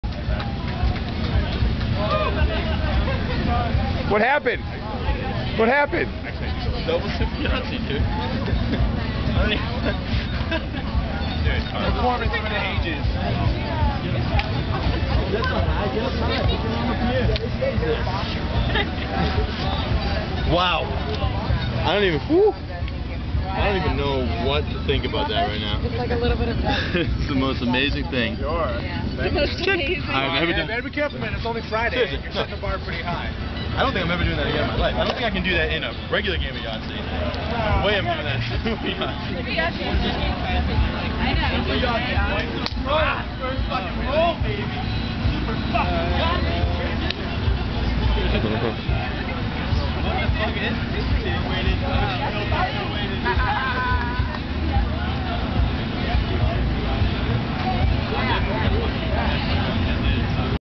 Always Go For Yahtzee - Super Yahtzee at Superball 7-1-11
Not only was the first game of Super Yahtzee this world has ever seen played at Superball...but the very first turn of that very first game resulted in the very first Super Yahtzee ever.  Epic moment?  Think again.  The very next turn he did it again.  That's right folks.  Two turns, two Super Yahtzees at the first Super Yahtzee game ever played, at the Superball.  If that's not Super I don't know what is.  The moral of this story?  ALWAYS go for Yahtzee.